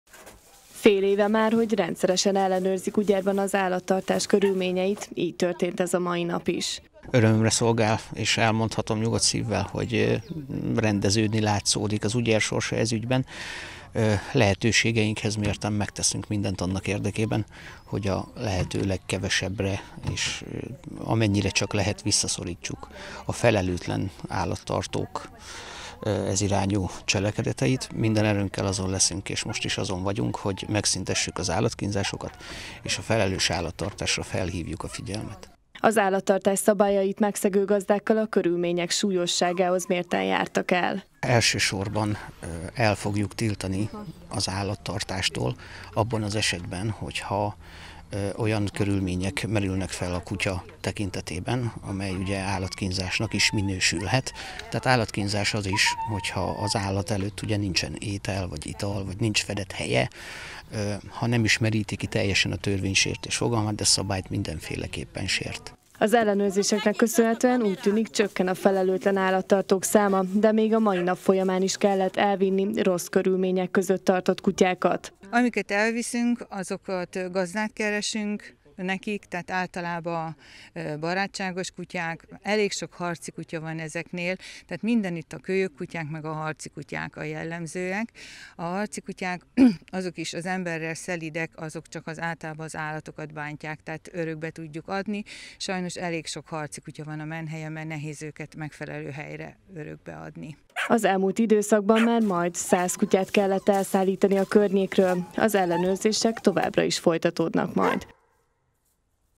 Újabb ellenőrzést tartottak ma az Ugyerban
Újabb ellenőrzést tartottak ma az Ugyerban. A rendőrség, a hatósági állatorvos, az önkormányzat és az állatmenhely együtt kereste fel az állattartókat. A rendszeres ellenőrzéseknek köszönhetően csökkent a felelőtlen állattartók száma az Ugyerban – mondta a körzet képviselője, Horváth Gábor.